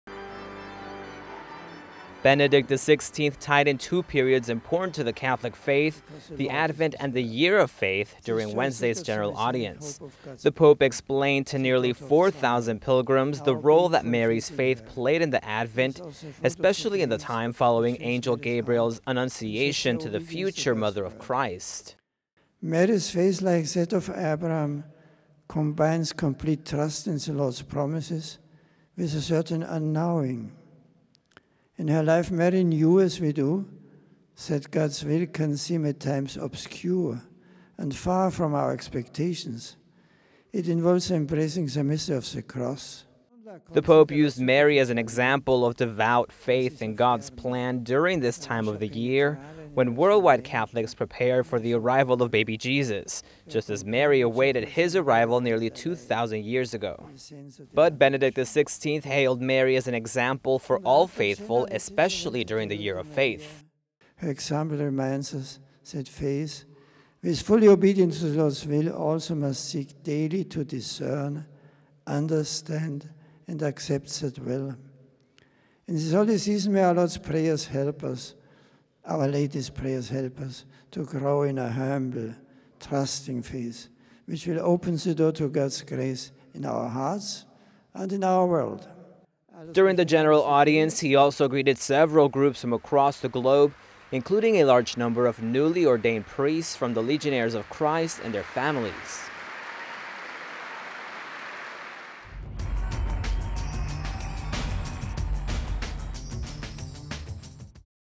Pope highlights Mary's faith in God in advent of Nativity
Benedict XVI tied in two periods important to the Catholic faith, the Advent and the Year of Faith, during Wednesday's general audience.